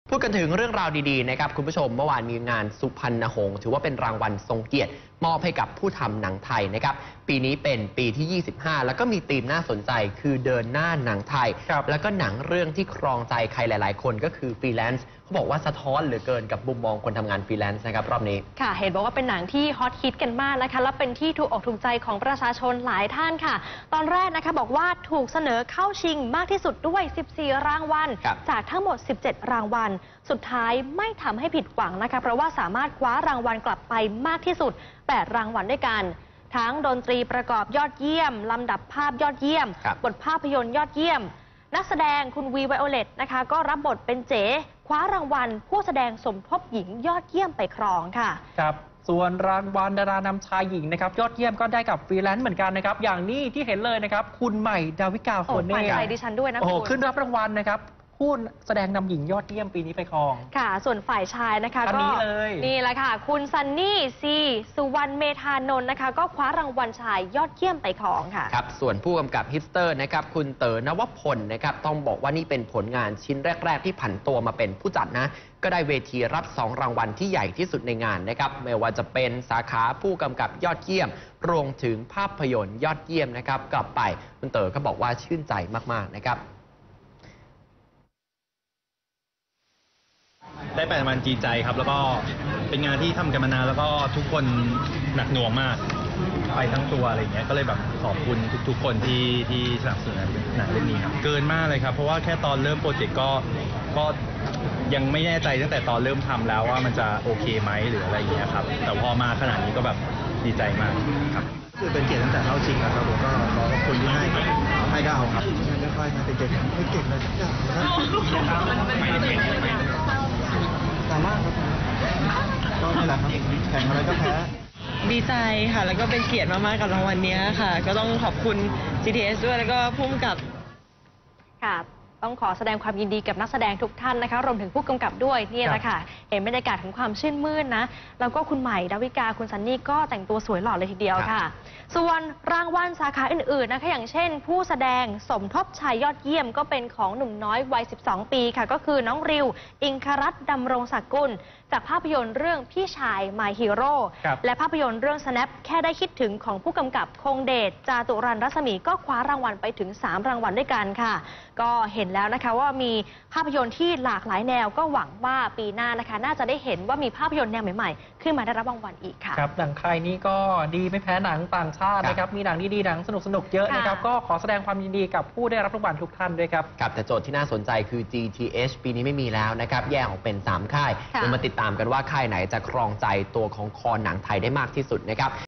News งานประกาศรางวัลสุพรรณหงส์ ครั้งที่ 25
พิกัดข่าวเช้า : งานประกาศรางวัลสุพรรณหงส์ ครั้งที่ 25 "ฟรีแลนซ์ฯ" คว้ารางวัลมากที่สุด 8 รางวัล "NOW26" 14-3-59